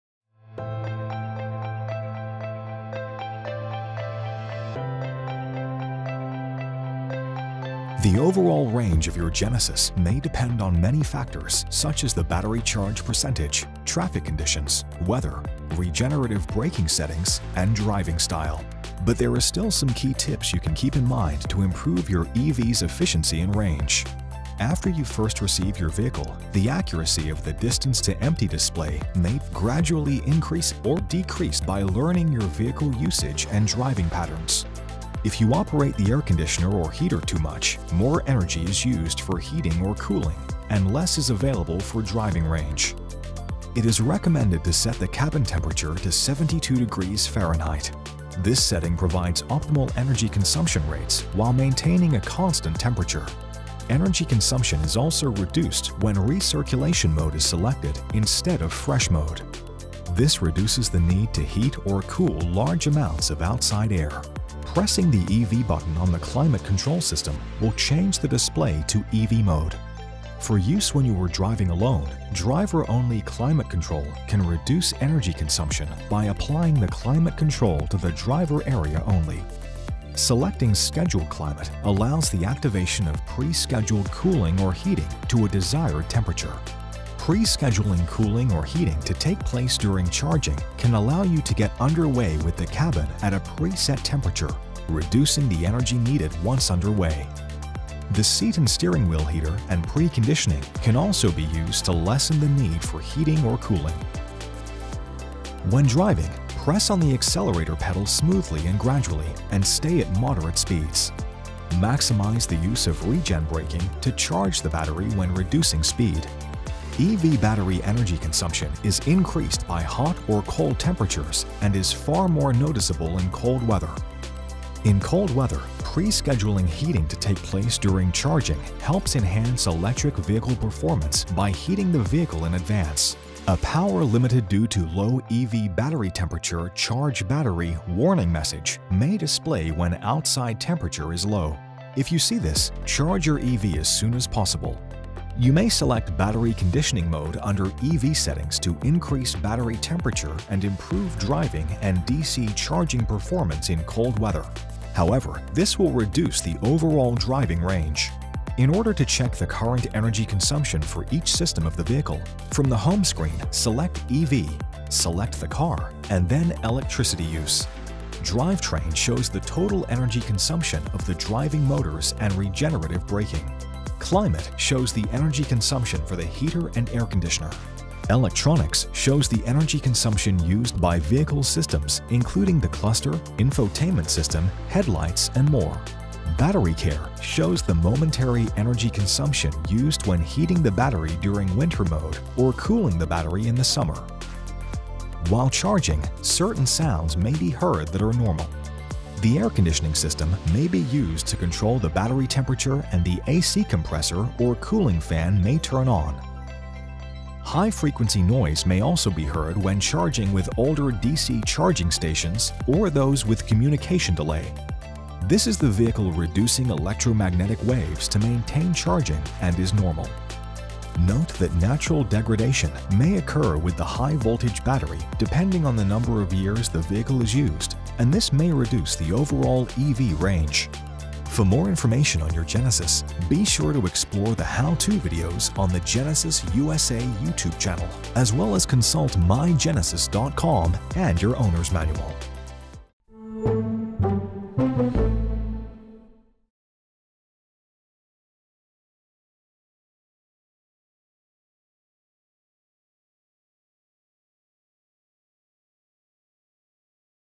Top Tips for Maximum EV Range | Genesis Electrified G80 | How-To | Genesis USA
Maximize your electric vehicle's driving range with these best practice efficiency tips to conserve energy and drive smoothly. Learn the top tips for maximum EV range on the Genesis Electrified G80.

Applicable models (if equipped):
2023+ Electrified G80